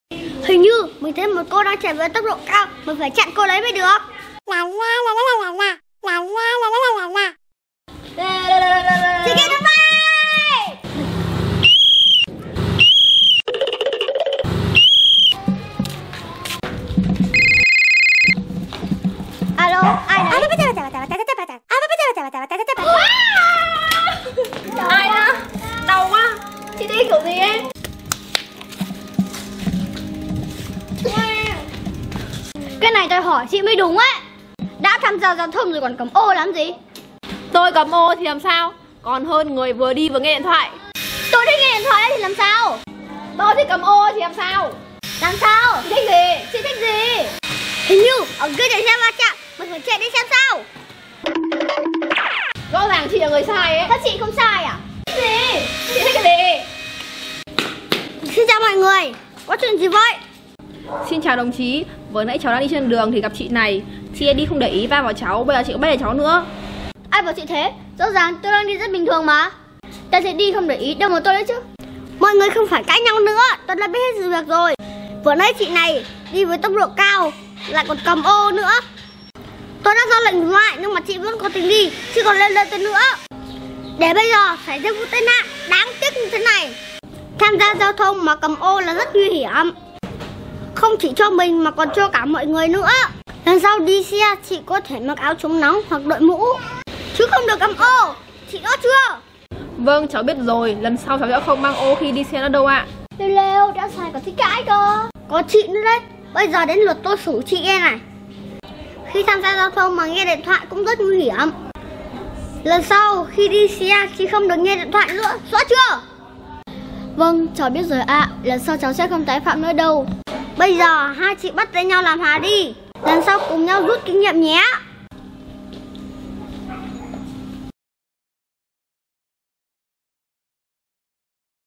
Chú công an giao thông tí hon - Dạy bé không nghe điện thoại khi tham gia giao thông
1 thông điệp nhóm mình xin gửi đến mọi người: Khi tham gia giao thông hãy tuân thủ luật giao thông nhé.
Đừng quên đăng ký và bấm chuông để nhận thông báo video mới nhất nha các bạn!
© Copyright by GT Vlogs - Play And Learn ☞ Do not Reup